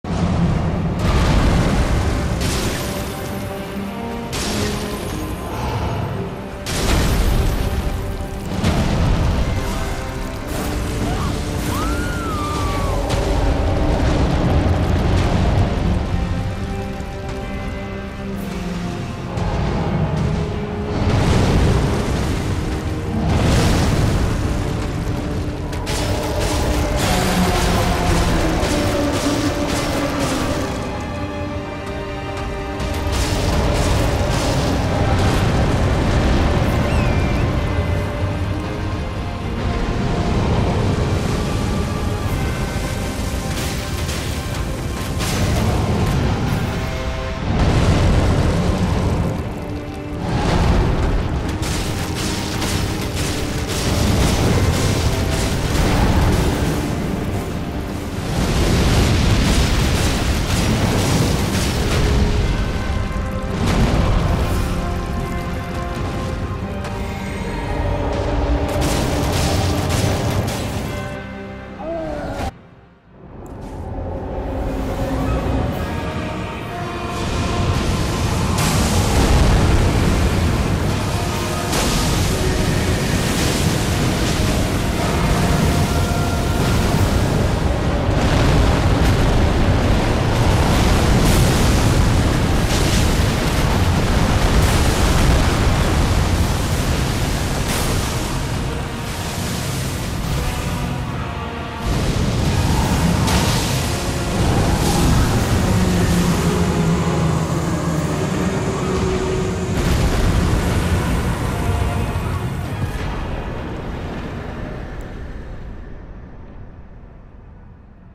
Turning Fire Giant into a porcupine (no hit, solo) Bow build RL76
The targeting for rain of arrows shifted by itself at the end of the video. I almost celebrated too early lol.

thank God for that last bit of mana to cast rain of arrows again

I used dwelling arrows because fire giant has lower resistance to magic